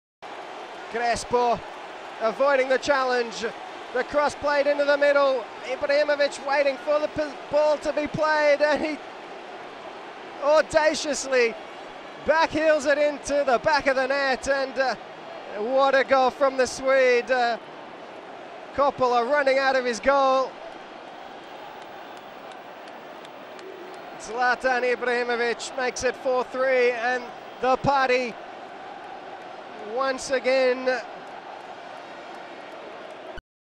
Ibra - goal against Atalanta 2009
Zlatan Ibrahimovic - goal against Atalanta 2009, allowing him to win the Serie A top scorer for 2008/2009